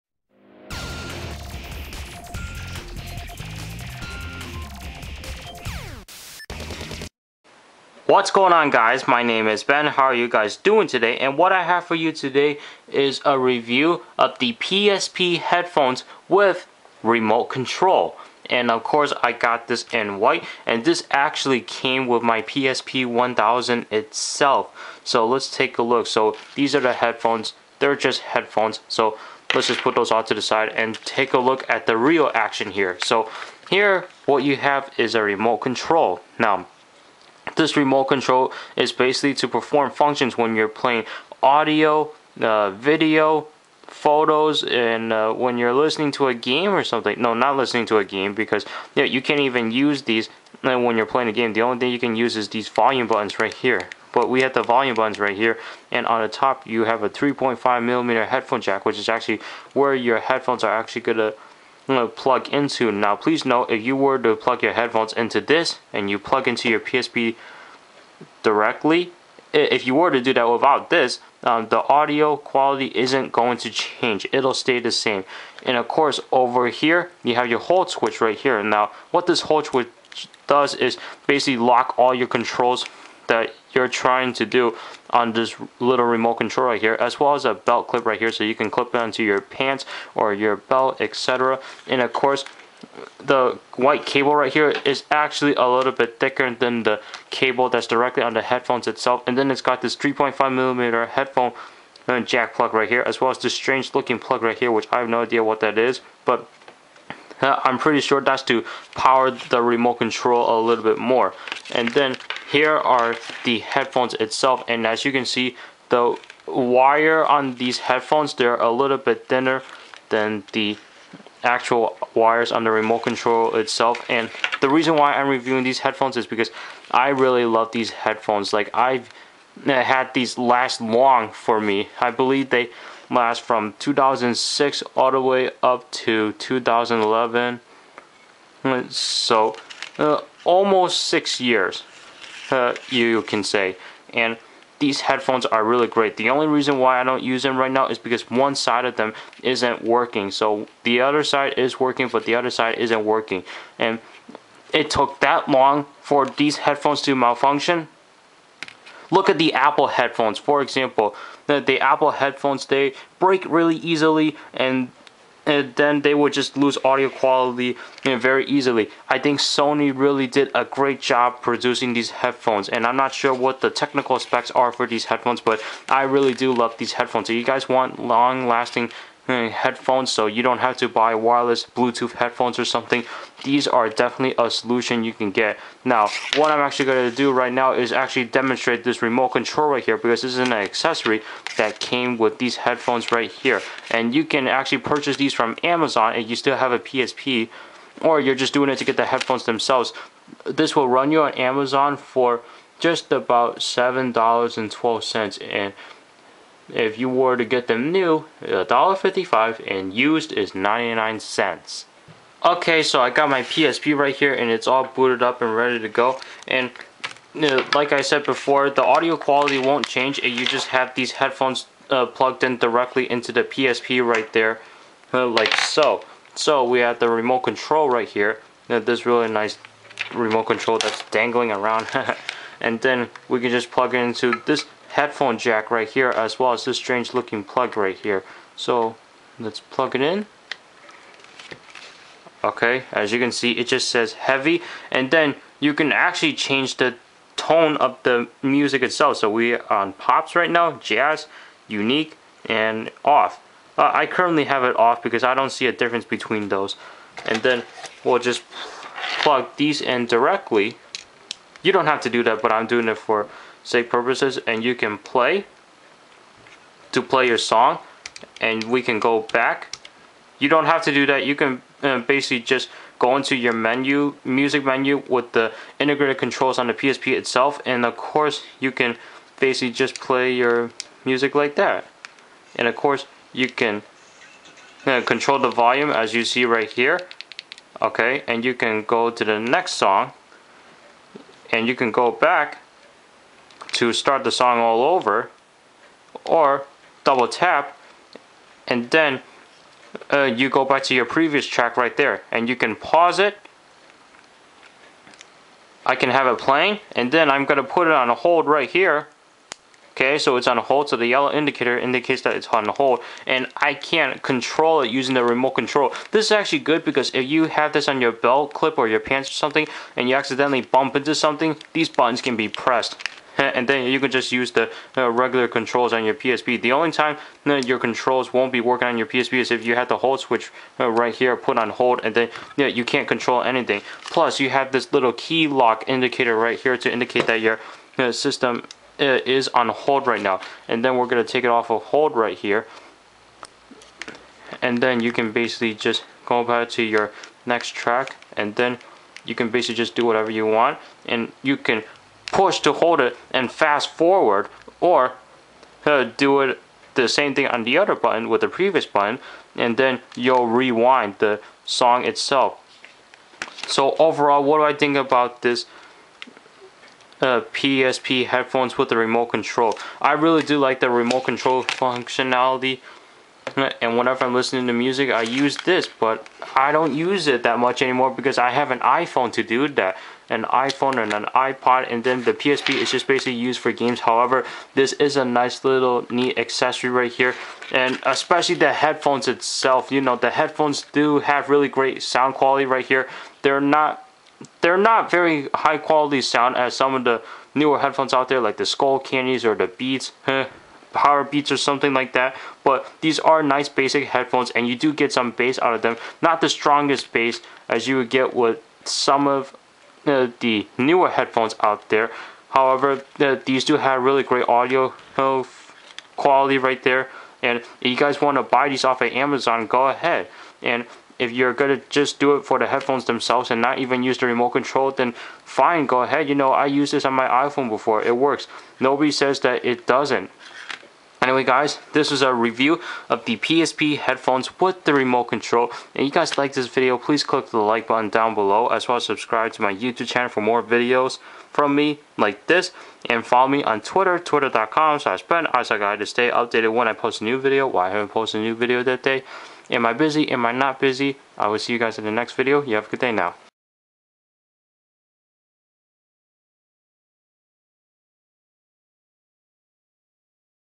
Review: PSP Headphones With Remote Control
Looking for some new headphones because your old pair already went down? What set of headphones can you get that will last you for awhile? Take a look at what I have to say for the PSP Headphones With Remote Control.